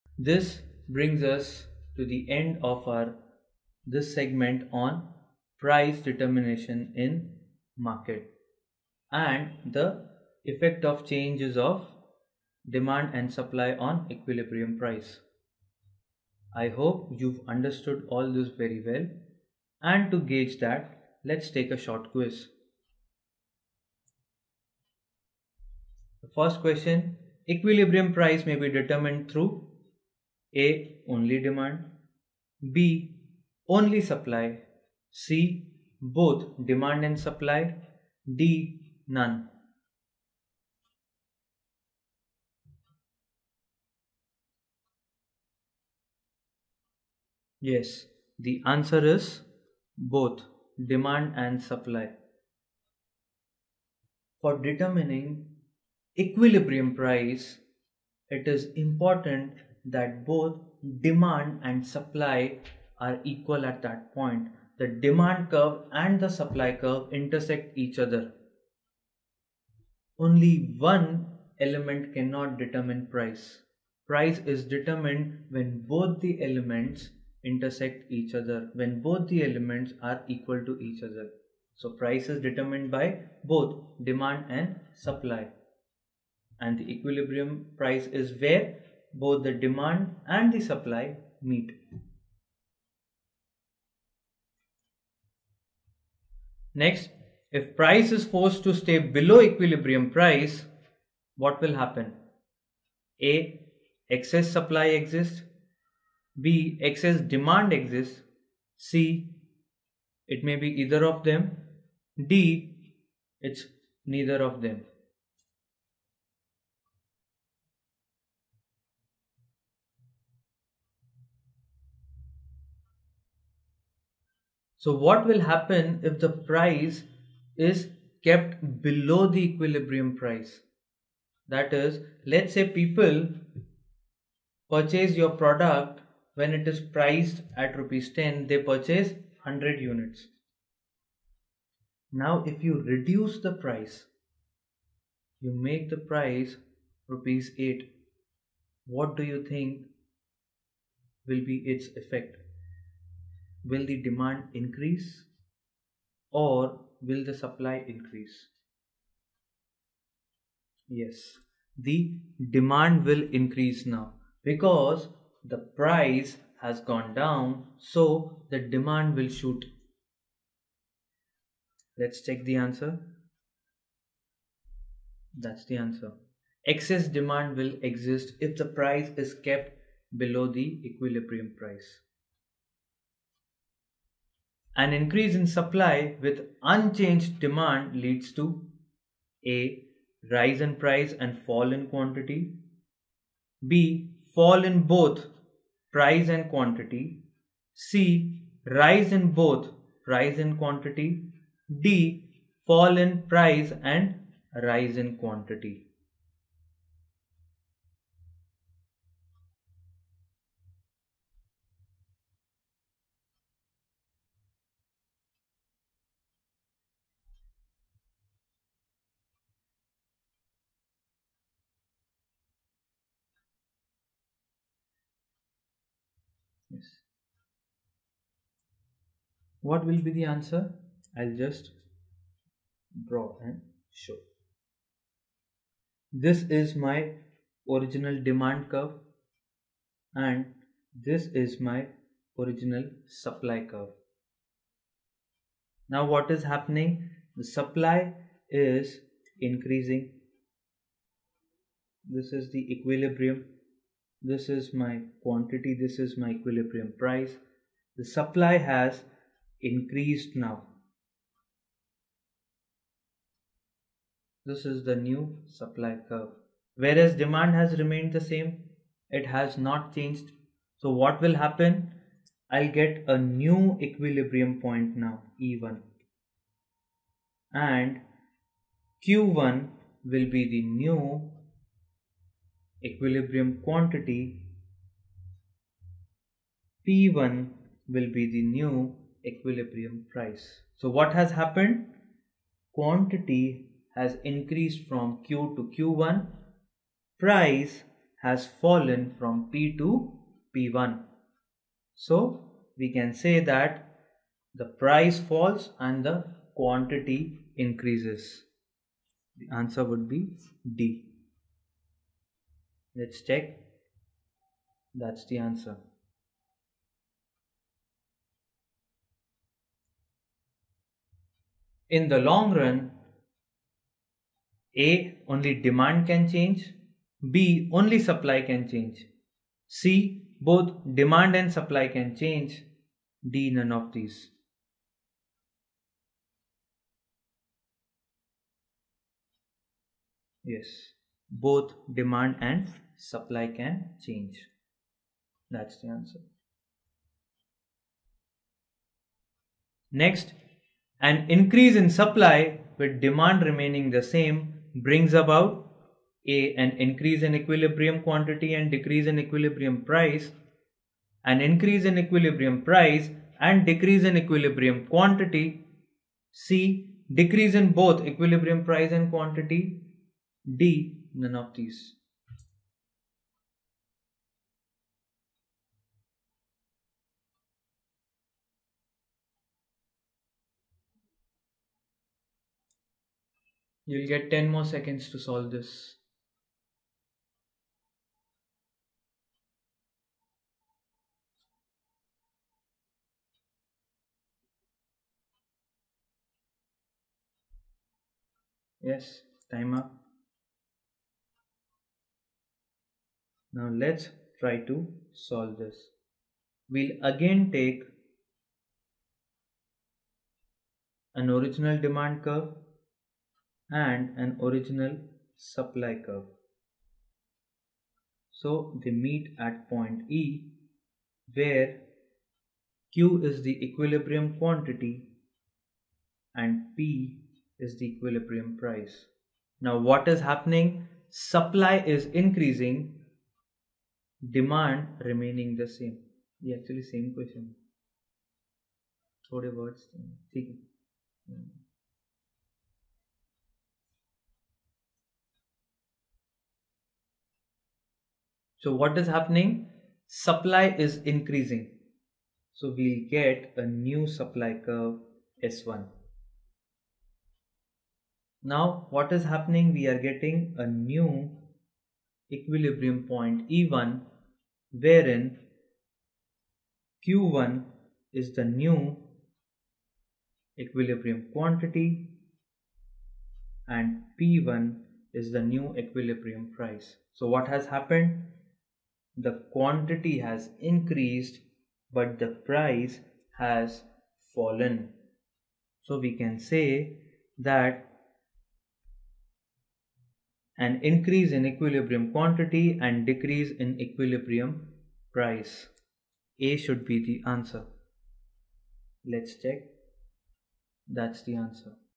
Problems and Solutions | Market | CA CPT | CS & CMA Foundation | Class 11 | Class 12
Quiz on Market, Learn What is Market, How Price is determined in the market?, How is Equilibrium reached with Change in demand and Supply.